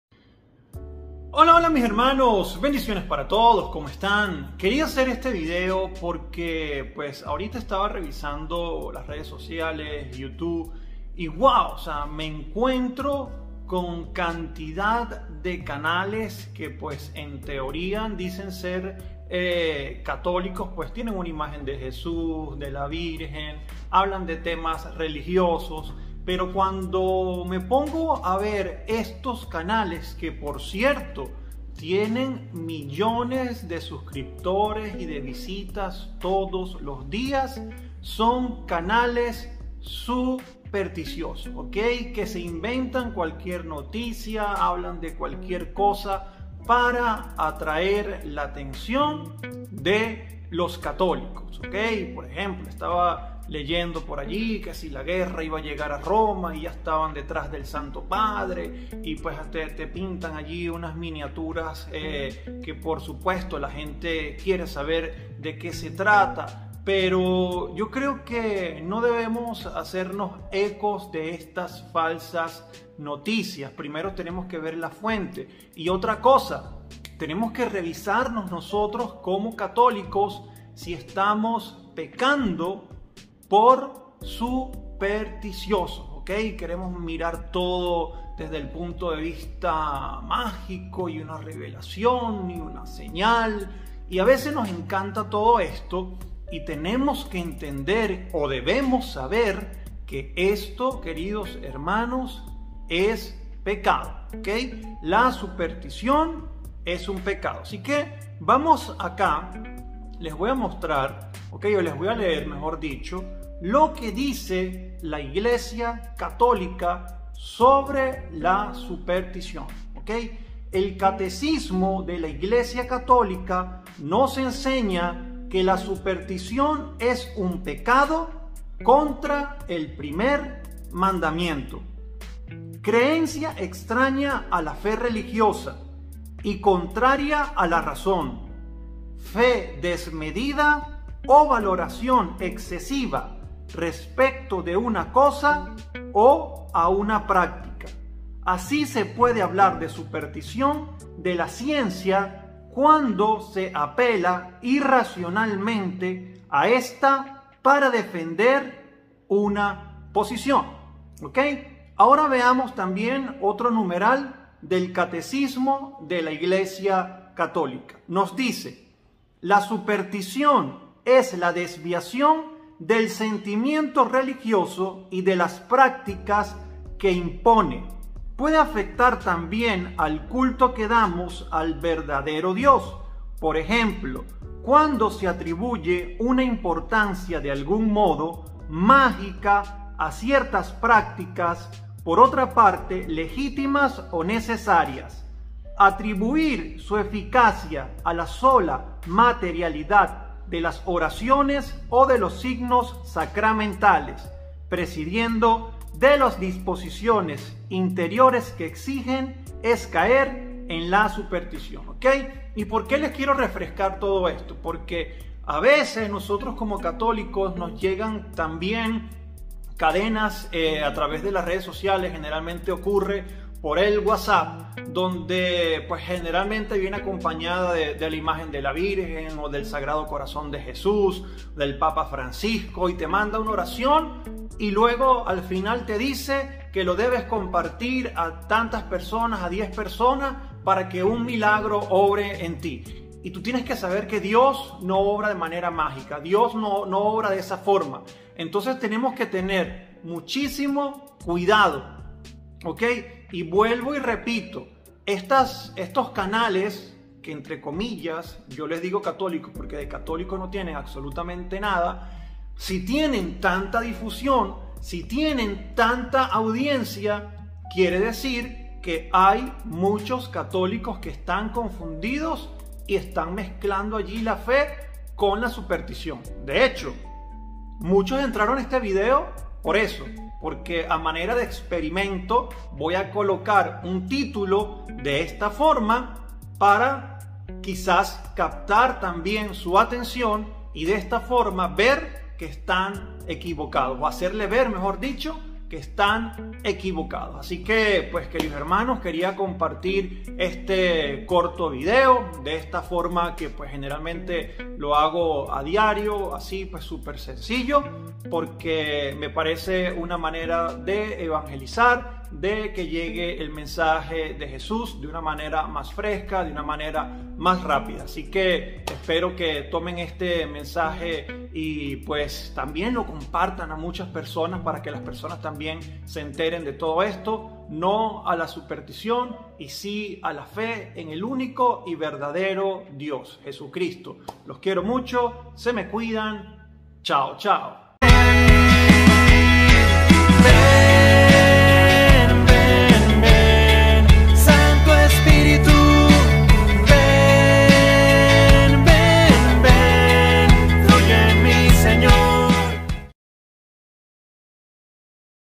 LA CAÍDA DEL PAPA FRANCISCO, SE CUMPLE PROFECÍA 🤯😱
Este es un vídeo que hice para reflexionar, ESPERO LO VEAS Y LUEGO ME DAS TU OPINIÓN.